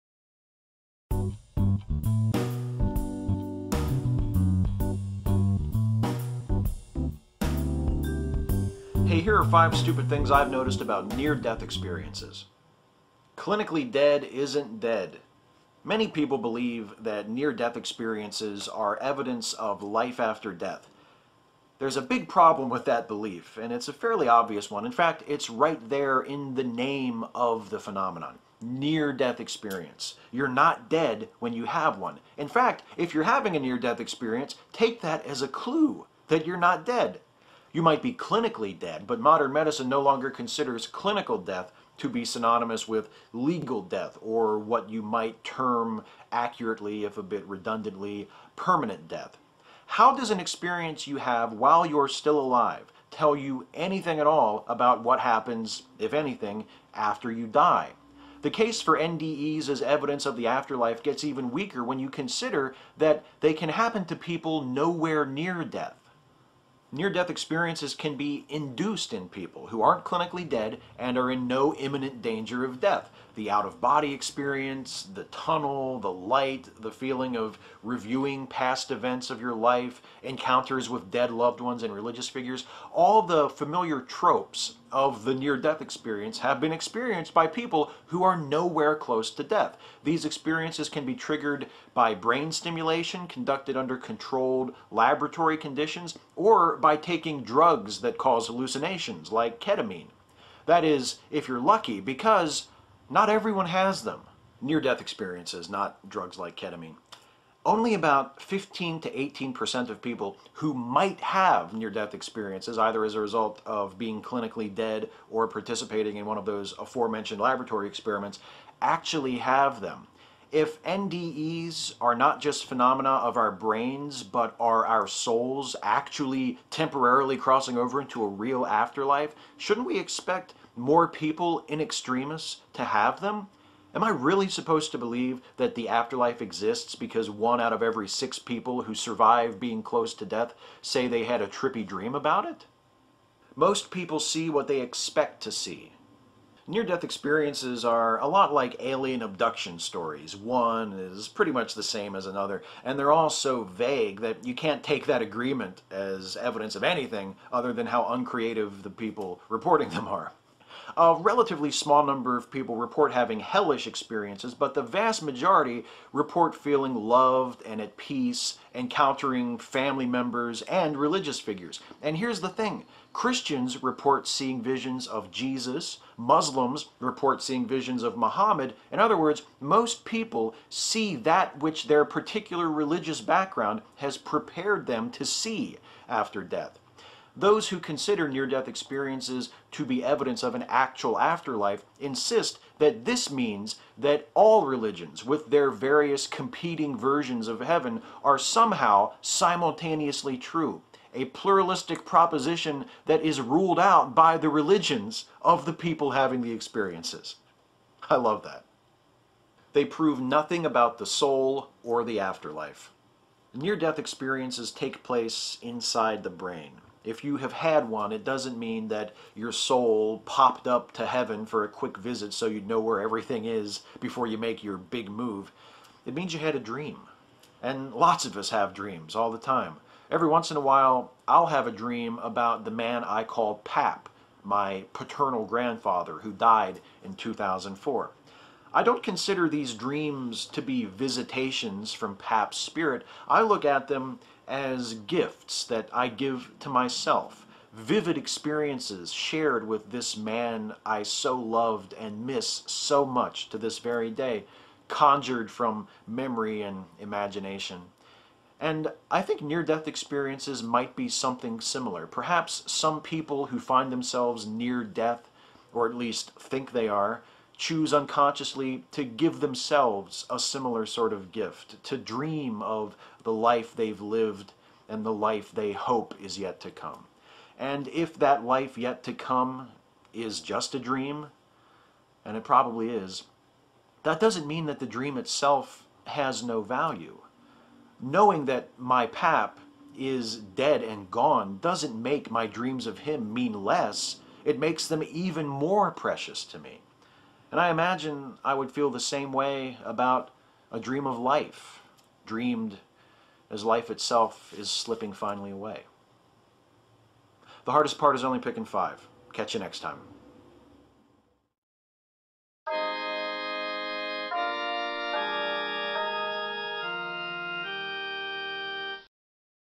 Five Stupid Things About Near-Death Experiences
What if you were in the tunnel, you walked into the light, and it turns out the light was a big screen playing this video? Would you think you'd gone to Heaven, or Hell?